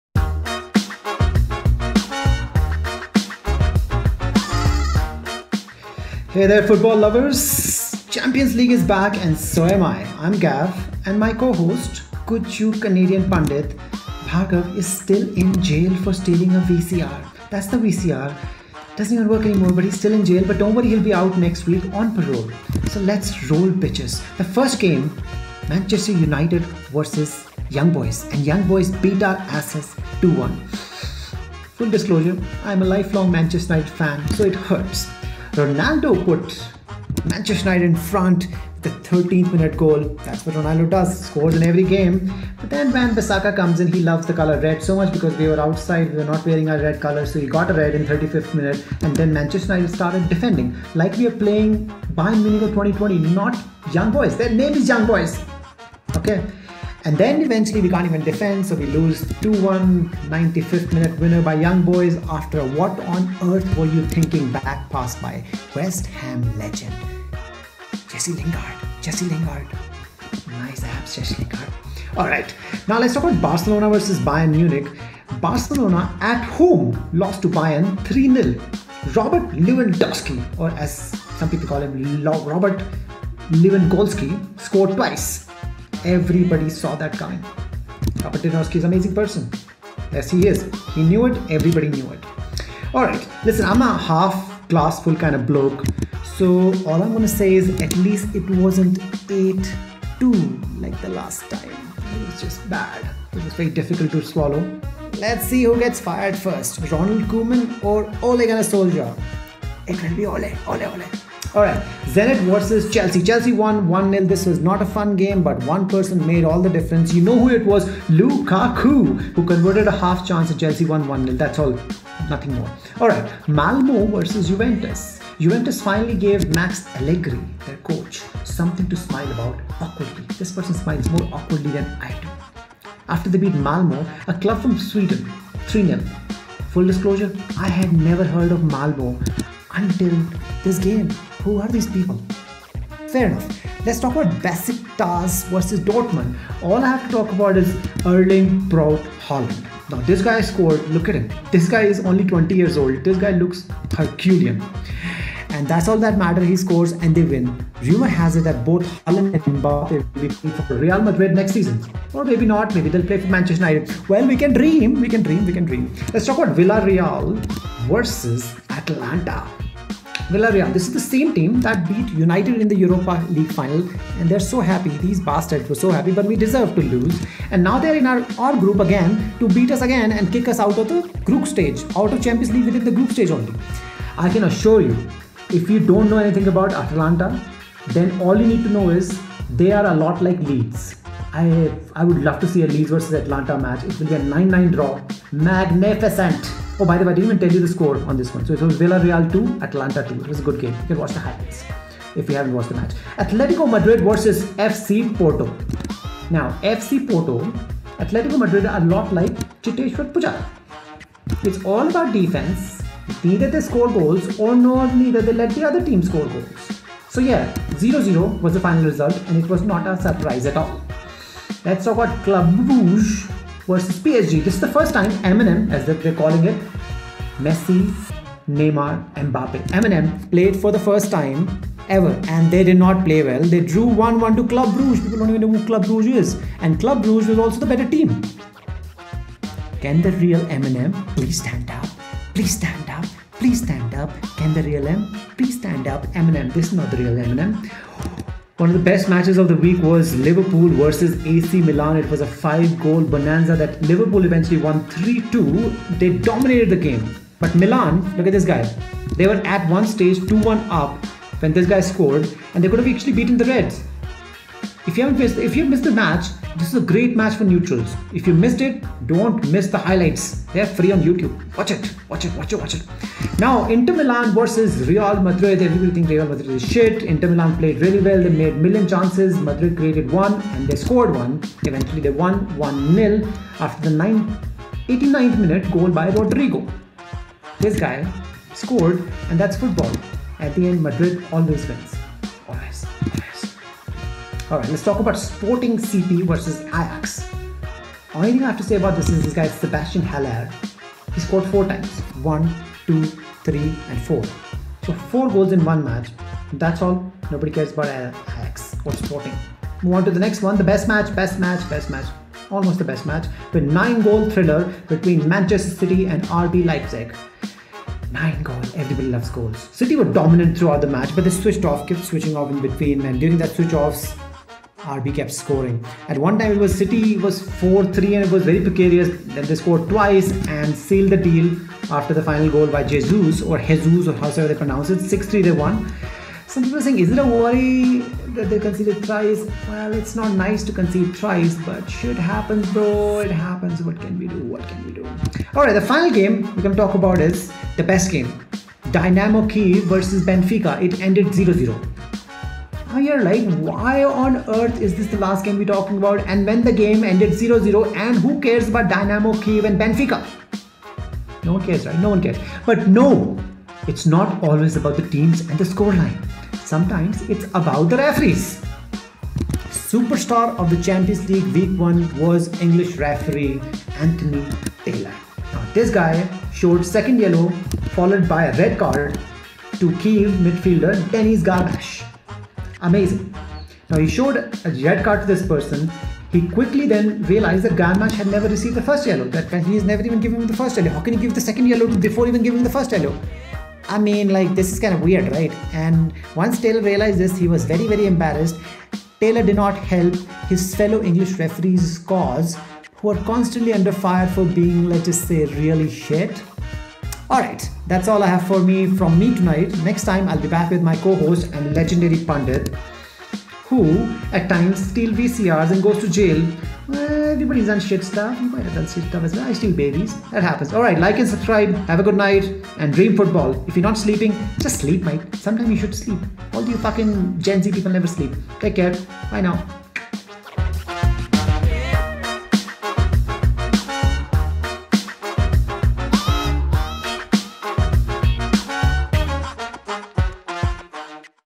Champions League — Week 1 — Review — United & Barcelona THRASHED!
Hey there, Football Lovers

Champions League is Back — So am I
— I’m Gav —
— My co-host — Gujju-Canadian Pundit —Bhargav [Picture] is still in Prison for stealing a VCR [Picture]

Anyway, he will be out next week —on parole

Let’s roll, bitches!

Young Boys  2 — 1  United

Young Boys beat United 2-1 — sob.sob.sob

Full Disclosure: I’m a life-long Manchester United Fan — so it hurts!

Ronaldo [Picture] scored in the 13th minute to put United ahead — then Wan-Bissaka sees RED in the 35th & United start defending like they’re playing Bayern Munich not Youngs Boys [Switzerland] — of course, we can’t defend — we lose 2-1 courtesy 95th minute winner  — after a what-on-earth-were-you-thinking back pass by West Ham Legend — Jesse Lingard! [Picture]

If United doesn’t make it to the knock-outs, Ronaldo will get Ole Fired! [Picture]

Barcelona  0 — 3  Bayern Munich

Barcelona lost 3 - 0 at home to Bayern. Lewandowski [Picture] scored twice — everyone saw that coming!
— Listen, I’m a glass-half-full kinda bloke — So, I will say — at least it wasn’t 8-2 [Picture] like the last time.

Let’s see who gets fired first — Ronald Koeman or Ole [it will be Ole]

Chelsea  1 — 0  Zenit

It wasn’t a fun game at all but one person made all the difference —— Lu.ka.ku [Picture] converted the half chance & Chelsea won 1-0

Malmö FF  0 — 3  Juventus —

Juventus finally gave Max Allegri [Picture] some reason to smile — awkwardly — after they beat Malmö — a club from Sweden — Full Disclosure: I had never heard of Malmö before this game!

Besiktas  1 — 2  Dortmund

Erling Braut Håland [Picture] scored & that’s all that matters —
Rumor has it that both Håland & Mbappé will be playing for Real Madrid next season or may be not — we will find out!

Villareal  2 — 2  Atalanta

Villareal — this is the Team that beat United in the Europa League final [Picture] & now they’re here to Beat United again — & kick them out at the Group Stage of CL
P.S. If you don’t know nothing about Atalanta — then all you need to know is that they’re a lot like Leeds — I’d love to see Leeds vs Atalanta — it’ll be a 9-9 draw — Mag.ni.fi.cent!

Atletico Madrid  0 — 0  FC Porto

Atletico Madrid are a lot like Cheteshwar Pujara [Picture] — it’s all about Defense — Neither they score goals — nor they let the other team score goals — so yeah, 0-0 was no surprise!

Club Brugge  1 — 1  PSG

M&M [Messi-Neymar-Mbappe] [Picture] played together for the 1st time & they didn’t play well — they drew 1-1 — and club Brugge were better!

Can the Real M&M please stand up! please stand up! please stand up!

Liverpool  3 — 2  AC Milan

5-goal Bonanza

Liverpool dominated this match but Milan [Picture] could’ve still beaten the Reds — great match to watch for the neutrals. If you missed it, don’t miss the highlights!

Inter  0 — 1  Real Madrid

Inter created million chances & Madrid created one & scored — & they won 1-0 with an 89th minute Rodrygo [Picture] goal —— that’s football — Madrid always wins!

Sporting CP  1 — 5  Ajax

Sebastian Haller scored 4 times — [Picture]

City  6 — 3  RB Leipzig

This was the Best match from the spectator’s POV — a 9-goal fiesta.
City were dominant, but they switched off, at times, during the game & at one time it was 4-3 before they scored twice & sealed the deal with 6-3 [Picture]
Is it a worry?It’s not nice to concede thrice, but shit happens, bro!

Dynamo Kyiv  0 — 0  Benfica

Dynamo Kyiv  vs  Benfica finished 0-0 — you’re like why are we talking about a 0-0 game that too between Dynamo Kyiv  &  Benfica — no one cares, right?

— It’s not always about the Teams & the Scoreline.

Sometimes it’s about the Referees

Superstar of CL Week One was English Referee — Anthony Taylor
— who showed the 2nd Yellow followed by a RED to Kyiv midfielder Denys Garmash. [Picture]
— Taylor quickly realized that Garmash had never received the 1st Yellow
— I mean you can’t give the 2nd Yellow to someone without giving them the 1st Yellow. Can ya? [Picture]

Anthony Taylor didn’t help his fellow English referees cause, who are constantly under-fire for being —really bad—

Alright — that’s all from me tonight — Next time, I will be back with my co-host & Legendary Pundit, who, at times, steals, VCRs.

Watch or Listen (Audio only links below) to the podcast to find out more.